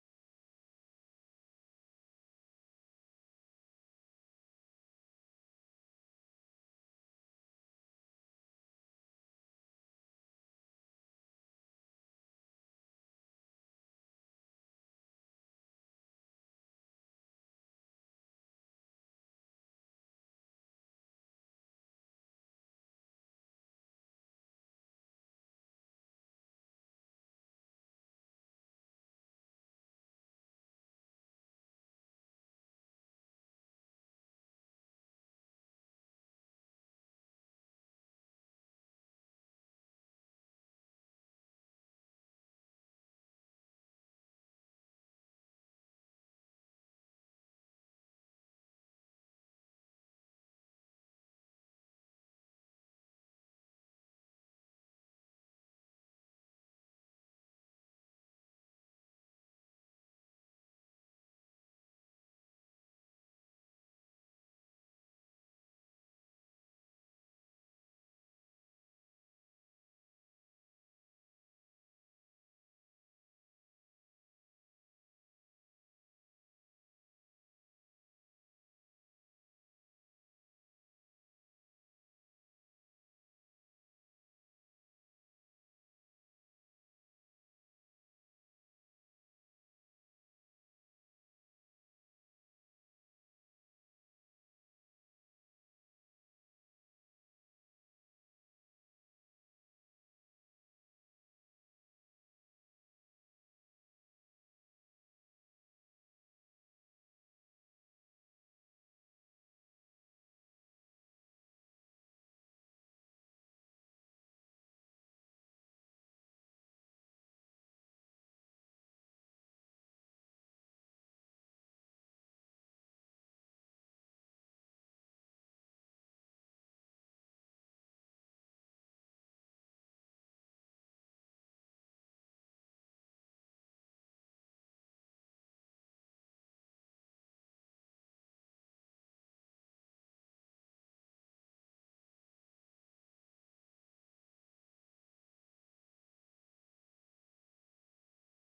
How to Select a Lottery Winner in Excel
Learn how to randomly select a winner for lottery in Excel.